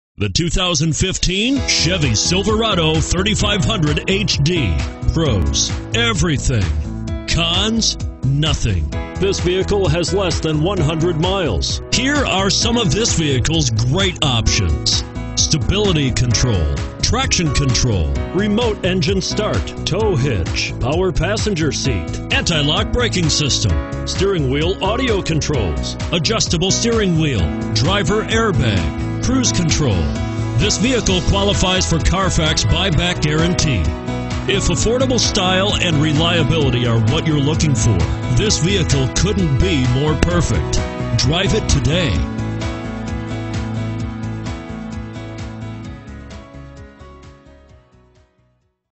2015 Chevrolet Silverado 3500HD Colorado Springs, Denver, Castle Rock 15008T
2015 Chevrolet Silverado 3500HD LTZ

230 N. Academy Blvd

You'll love this 2015 Chevrolet Silverado 3500HD. This is a car you'll want to take home. With 5 miles, it features Automatic transmission and an exterior color of Blue Granite Metallic. Call and get in touch with Al Serra Chevrolet VW directly, and be the first to open the car door today!